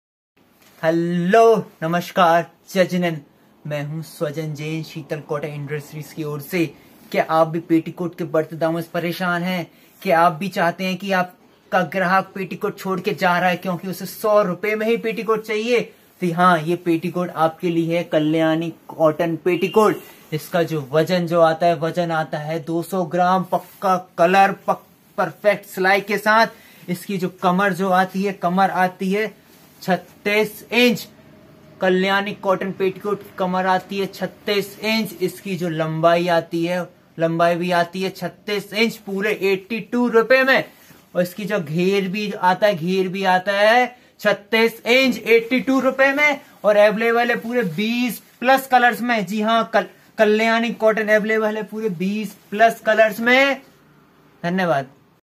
Kalyani Cotton petticoat| wholesale| ph: 9926384774 | MRP:99 👍
*order minimum 2 piece for personal use*
kalyani cotton petticoat has a little thick fabric has a huge demand in small cities has weight approx of 200 g .
available in all colours
no colour fade after washing(fast colour)
available in all 20 running colours
size : free size
waise: 38 inch
length: 38 inch
botton(gher): 38 inch